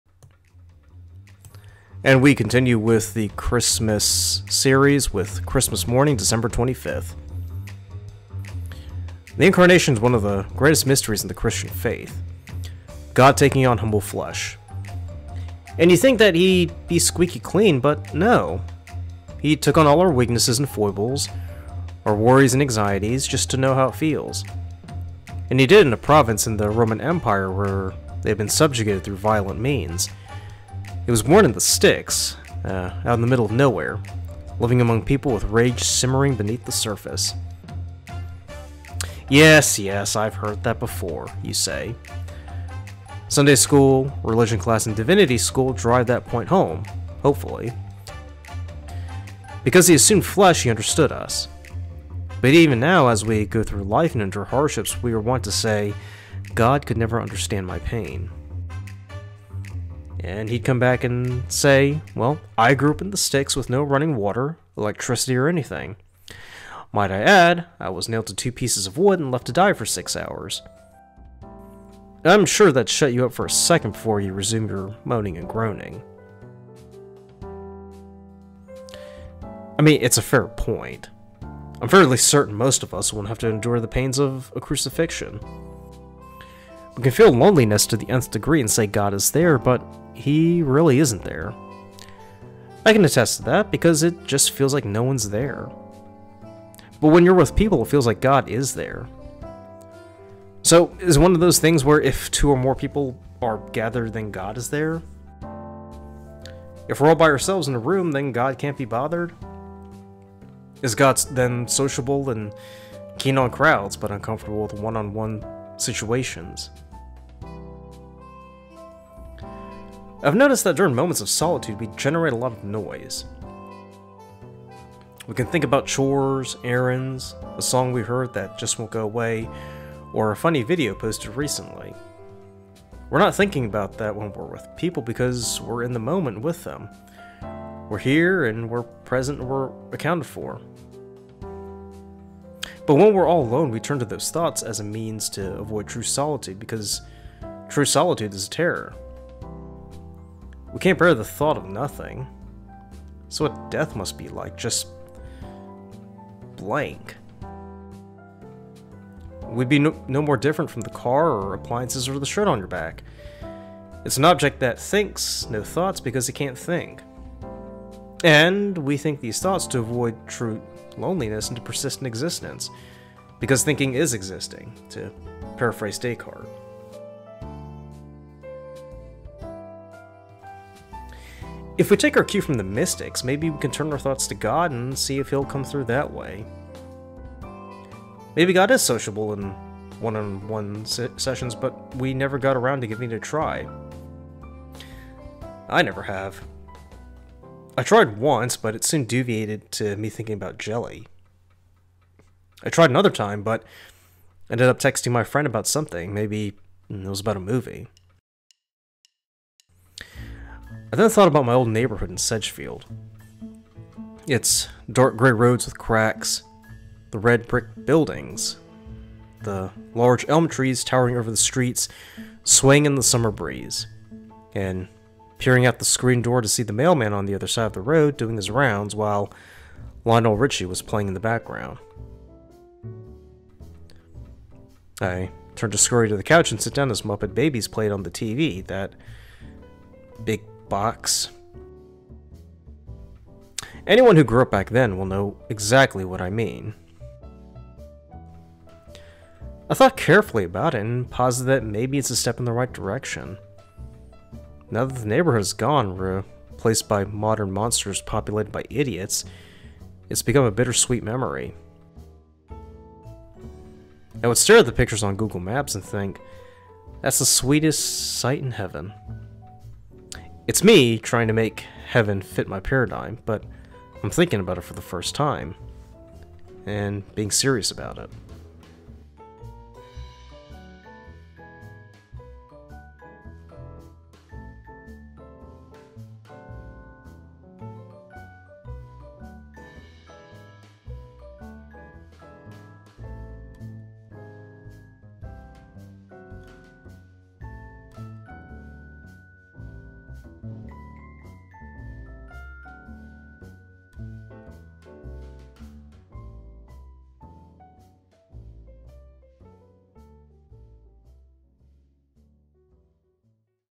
The Christmas Series: Christmas Morning
And Merry Christmas! This will be the first of three videos for Christmas Day. Today, I take a look at the mental state of the human species and wonder, "Are we actually ok or do we need to see someone?" I certainly think so but, again, that's just my humble opinion.